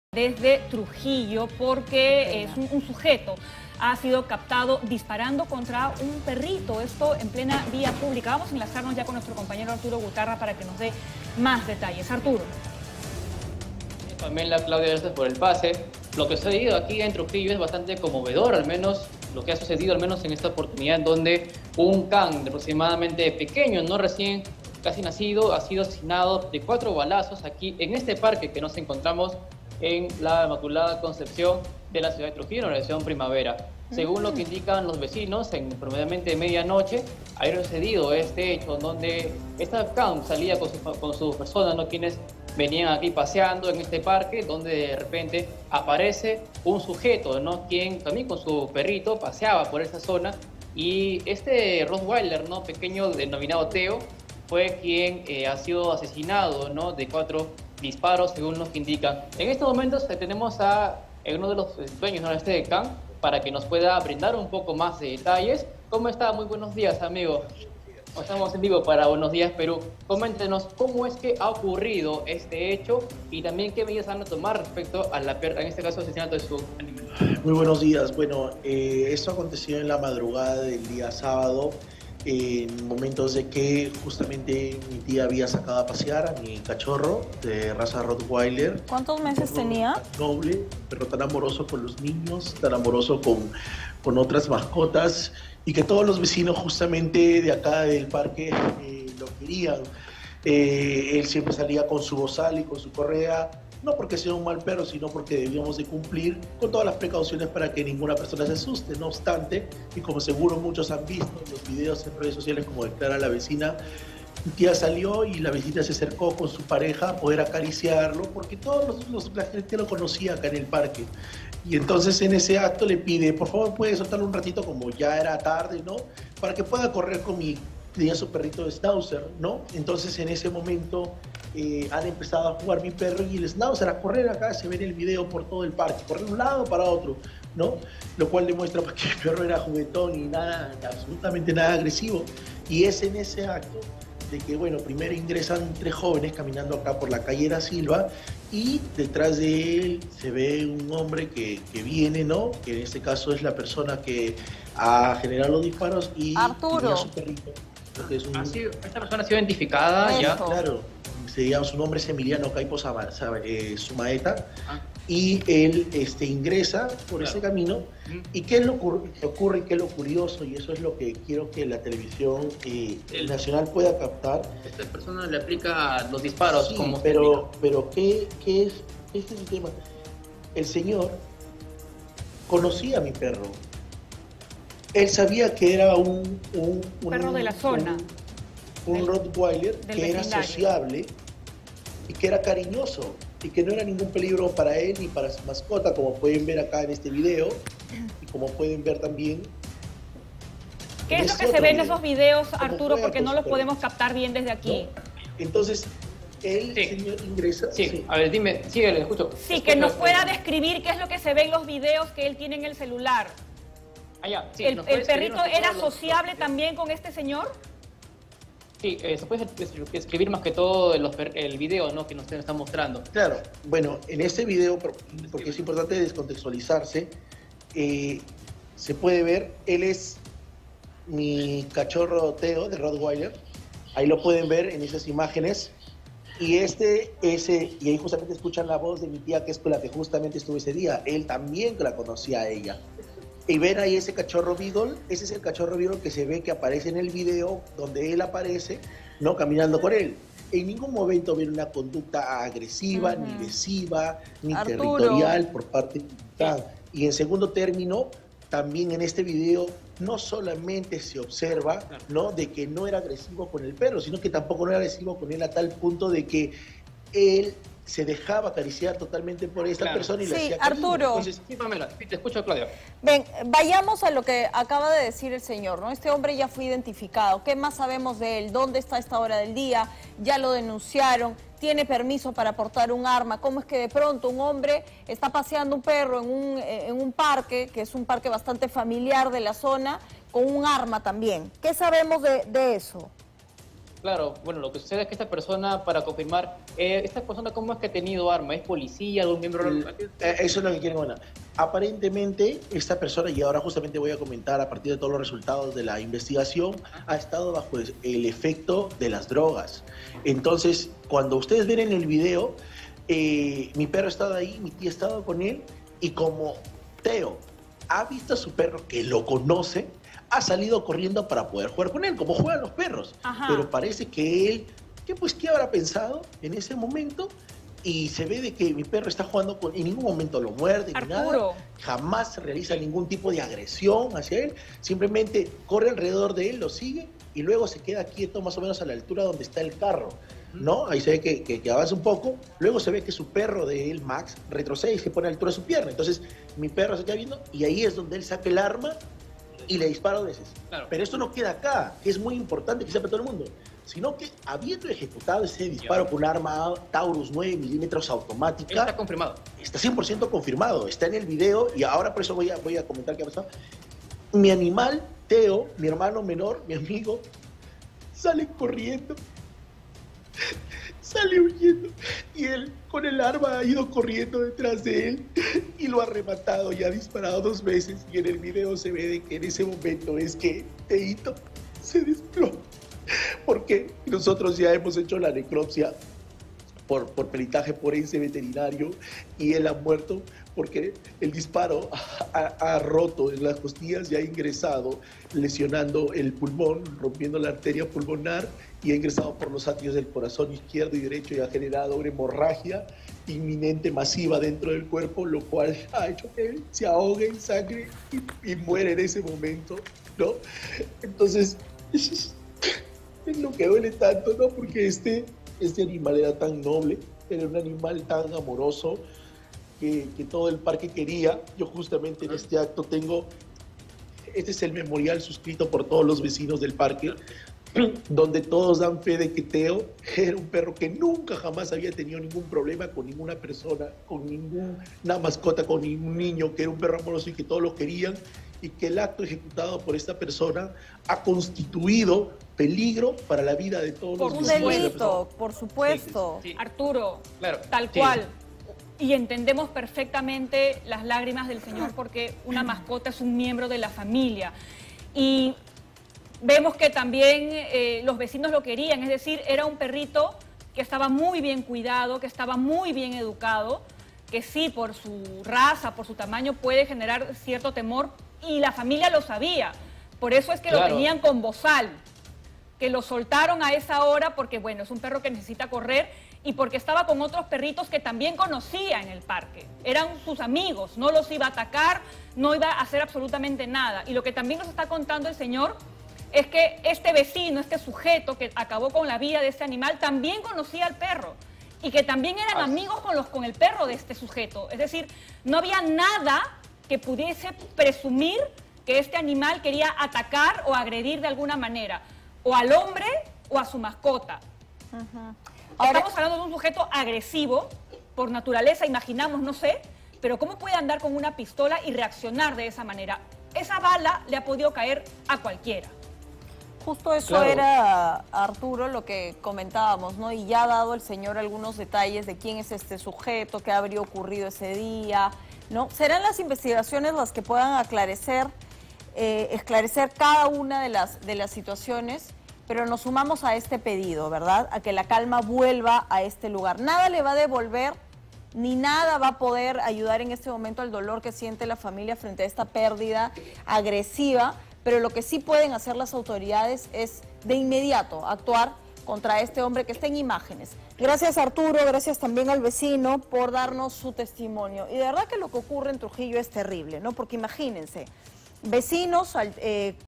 Justicia para Teo: sujeto dispara 4 veces contra perro que jugaba en parque de Trujillo
Un sujeto disparó contra un perro que se encontraba jugando en la urbanización Primavera, en la ciudad de Trujillo.

Teo era un cachorro de raza Rottweiler que había salido a pasear junto a su dueña al parque Inmaculada Concepción, cuando fue acribillado de cuatro balazos presuntamente por un hombre identificado cómo Emiliano Paico Zumaeta de 24 años de edad.

Luego del ataque, los responsables del perro indicaron que en todo momento tenía puesto su bozal y que era amigable con varios vecinos de la zona.

JUSTICIA PARA TEO

Hasta el lugar de los hechos, llegó una patrulla de la Policía Nacional para trasladar al atacante hasta la comisaría de el Alambre, donde pasó todas las diligencias correspondientes. Para poder excusarse de su cometido, Emiliano Paico indicó que actuó en defensa propia.

Las cámaras de seguridad captaron cómo Teo comenzó a perseguir, de manera juguetona, a la mascota del acusado por lo que tomó distancia y para evitar que se siga acercando, sacó su arma de fuego para acabar con la vida del can quien terminó tendido en el pasto. El dueño del animal fallecido señala que Paico conocía a Teo, y señala que presuntamente estuvo bajo los efectos de alguna droga.

Por ultimo, tras este lamentable suceso, el propietario del cachorro, junto a varios colectivos animalistas, realizaron un plantón en la Plaza de armas de Trujillo para exigir a las autoridades que este crimen no quede impune.